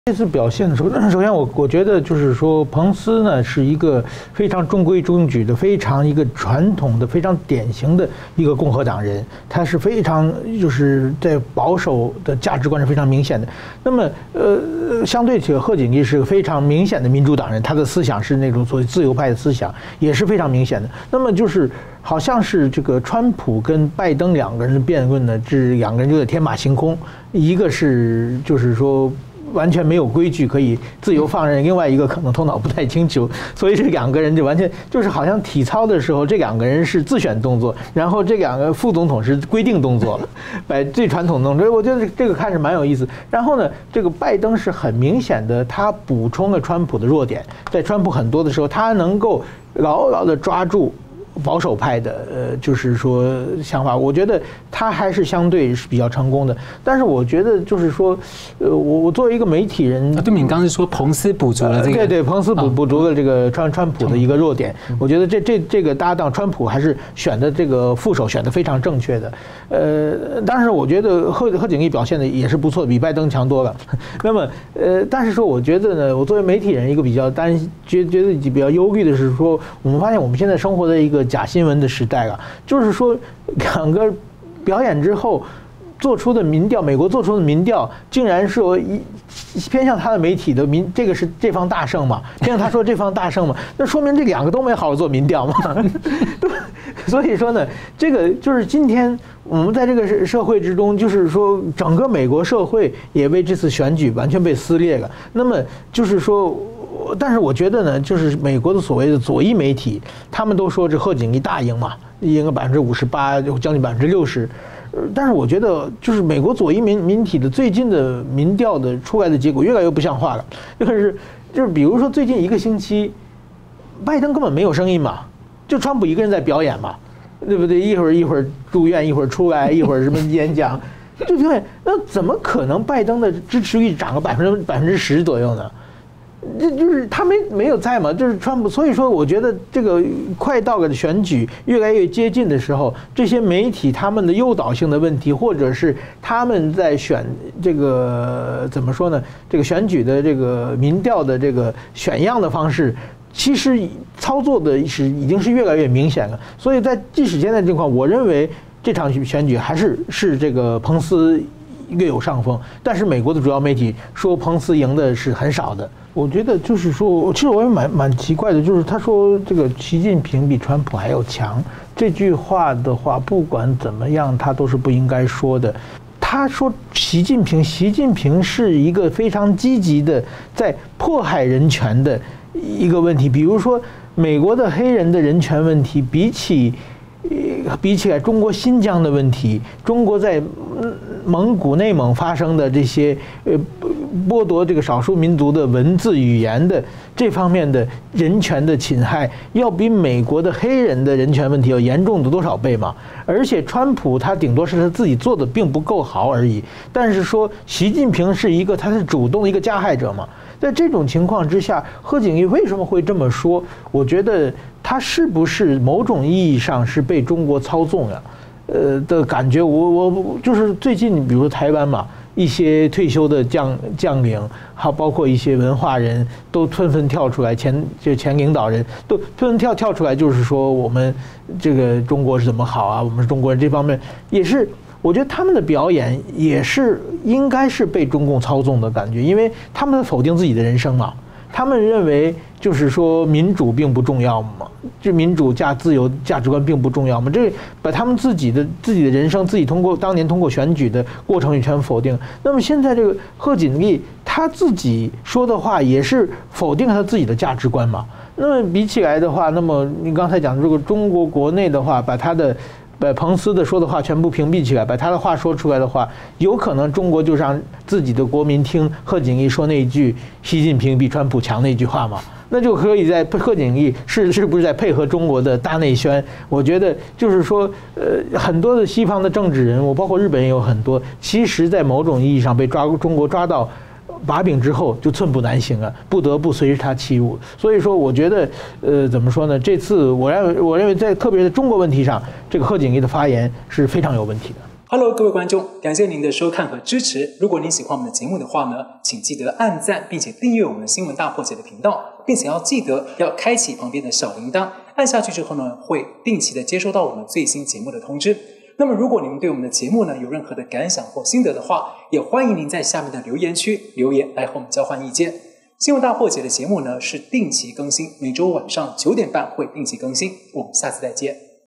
精彩片段》🔥副總統辯論中國問題大對決！賀錦麗助攻中共大內宣？｜矢板明夫｜@新聞大破解
美國總統大選川普拜登第二場辯論22日登場。7日，副總統候選人唯一場辯論，交鋒九大議題，彭斯抨擊中共的畫面，中國境內轉播中斷，到賀錦麗講話畫面就恢復；中共在當誰的豬隊友？
　皮尤對14國民眾做民調，對中共惡感創歷史新高。蓬佩奧帶著川普指示，出席四方對話，亞洲版北約成為焦點；他受訪表態要編織一個印太安全保障結構，對抗中共威脅。
　39國在聯合國指責中共迫害境內與香港。世界也關切，中共會不會趁機侵略台灣？蓬佩奧、美國國安顧問都警告中共，美軍也持續在日本西南演習。
　川普爭取連任之路，對手拜登之外，更明顯對手是中共。川普加速滅共腳步，召集印太盟友跟上，2020大戲，高潮迭起。邀請產經新聞台北支局長矢板明夫為您深度解讀。